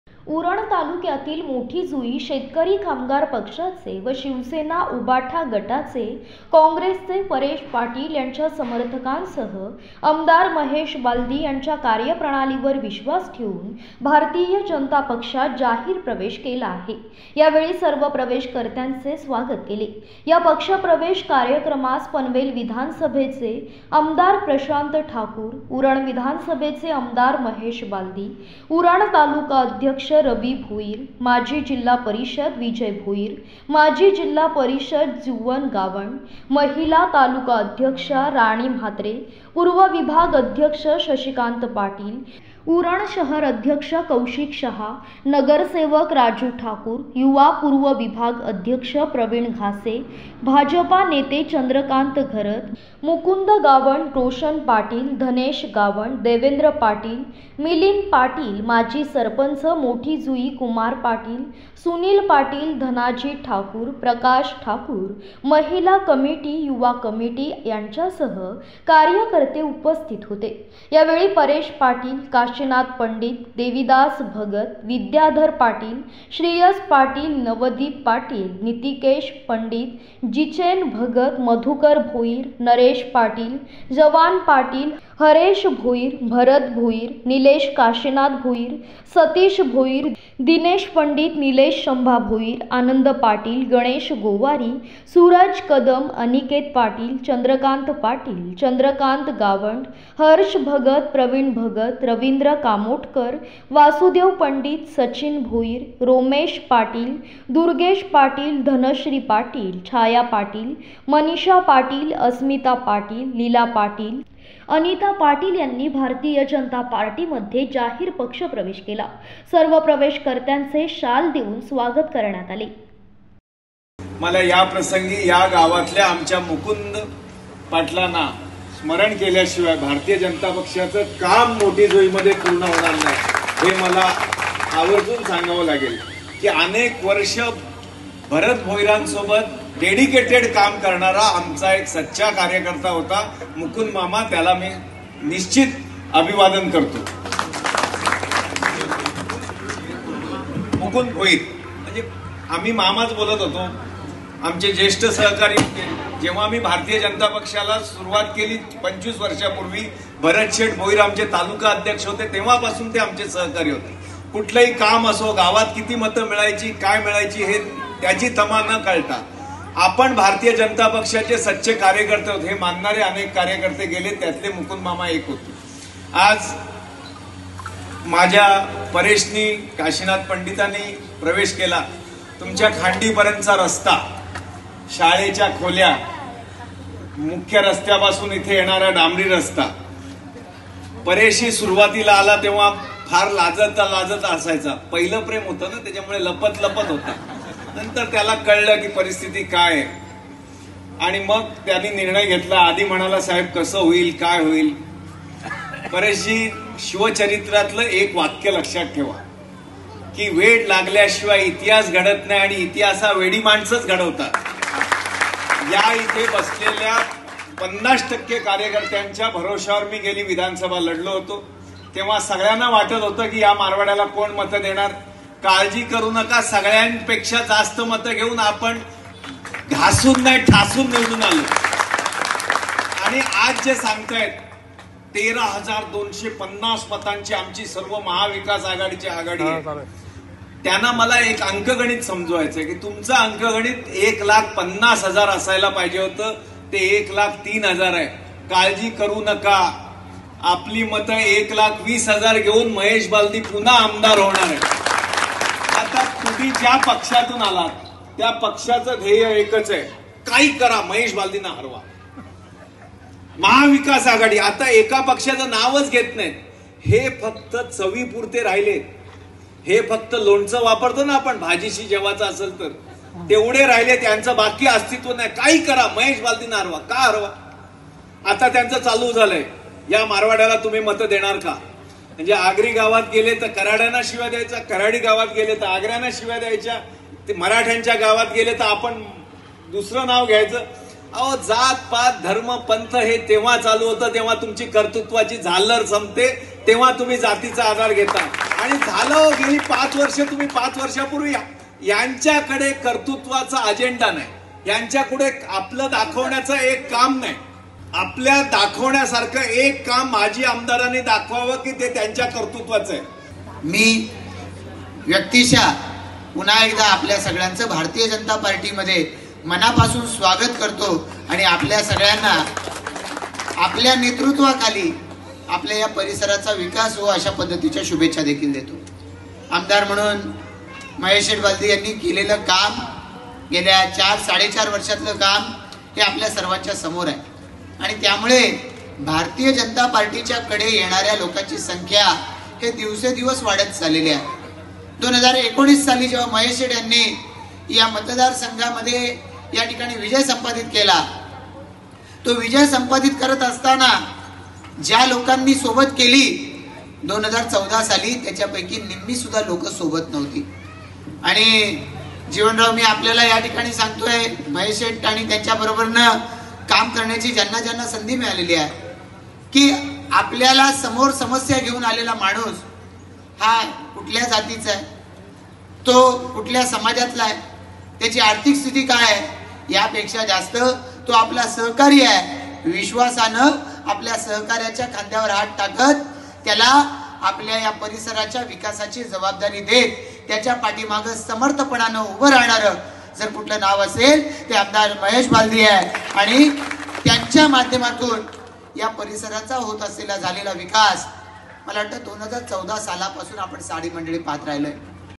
मोठी जुई येथील शेतकरी कामगार पक्षाचे व शिवसेना उबाटा गटाचे, काँग्रेस पक्षाचे कार्यक्रर्ते भाजपात !!
उरण तालुक्यातील मोठी जुई येथील शेतकरी कामगार पक्षाचे , शिवसेना ( उद्धव ठाकरे ) गटाचे,व काँग्रेस पक्षाचे कार्यक्रर्ते भाजपात#malharnews